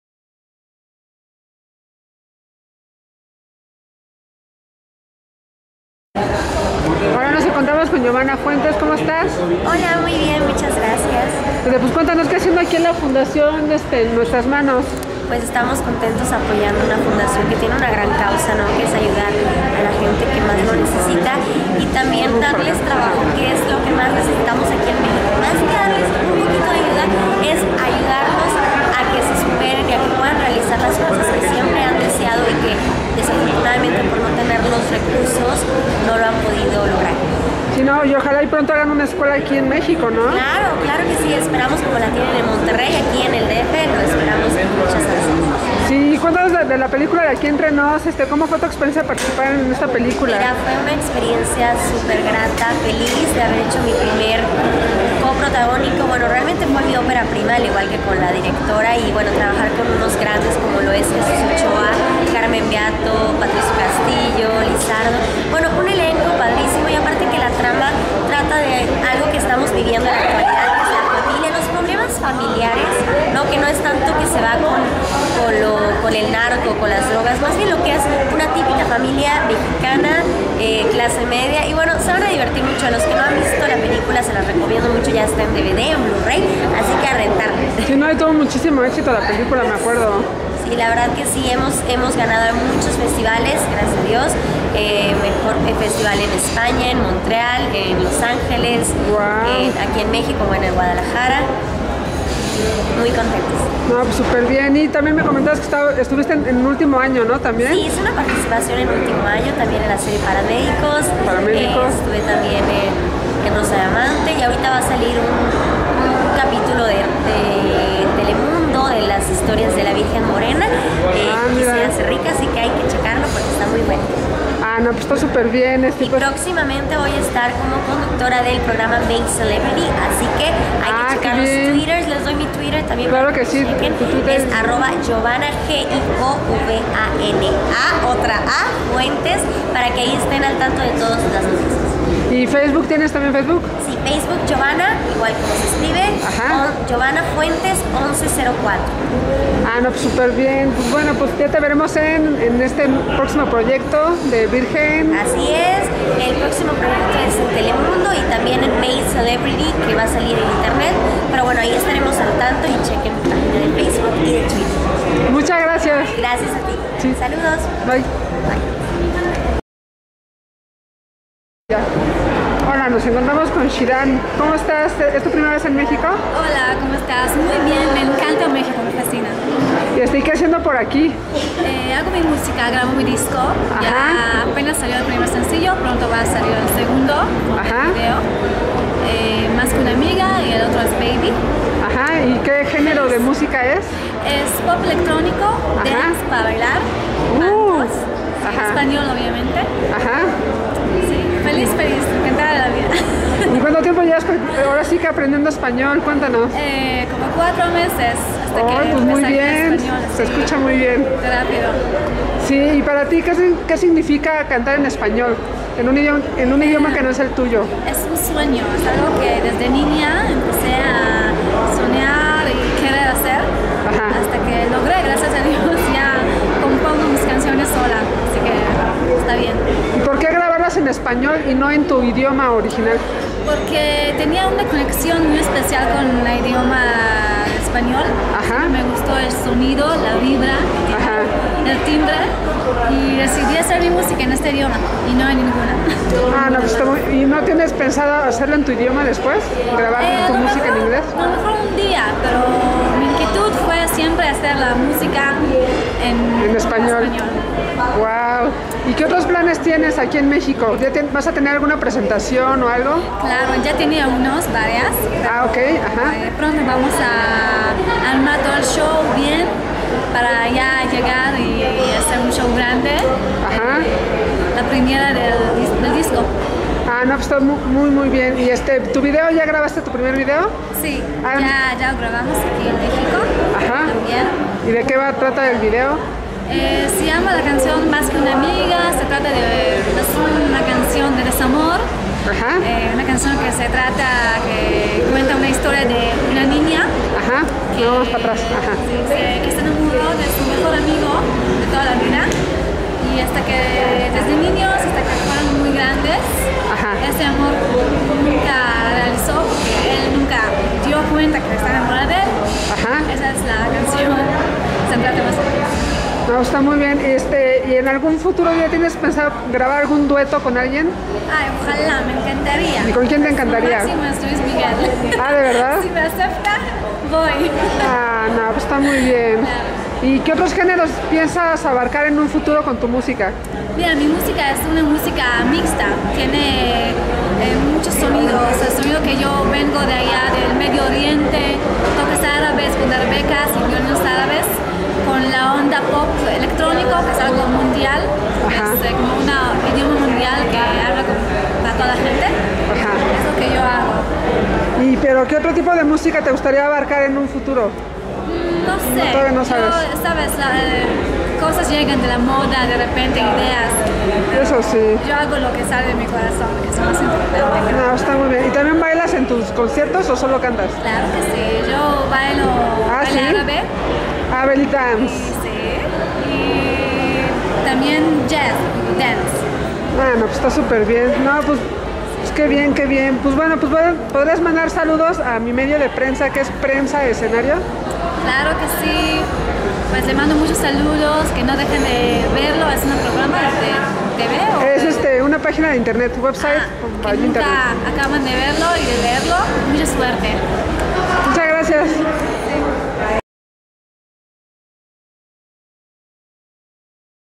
GIOVANNA FUENTES - SHIRAN - ENTREVISTAS
Entrevista con la actriz Giovanna Fuentes que participo recientemente en la película "Aquí entre nos" al lado de Jesús Ochoa. También entrevista con Shiran, talento nuevo y próximo lanzamiento musical.